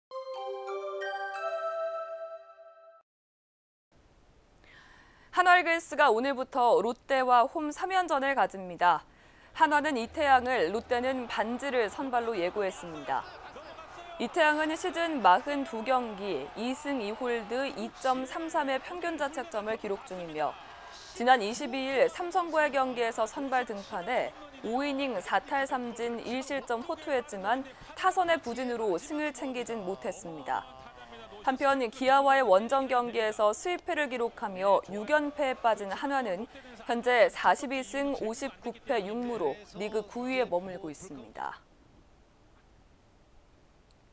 [대전뉴스] 한화이글스, 오늘부터 롯데와 홈 3연전... CMB 생중계
[아나운서]
한화이글스가 오늘부터 롯데와 홈 3연전을 가집니다.

[기사]
한화는 이태양을, 롯데는 반즈를 선발로 예고했습니다.

이태양은 시즌 42경기 2승 2홀드, 2.33의 평균자책점을 기록 중이며, 지난 22일 삼성과의 경기에서 선발 등판해 5이닝 4탈삼진, 1실점 호투했지만 타선의 부진으로 승을 챙기진 못했습니다.

한편 기아와의 원정 경기에서 스윕패를 기록하며 6연패에 빠진 한화는 현재 42승 59패 6무로 리그 순위 9위에 머물고 있습니다.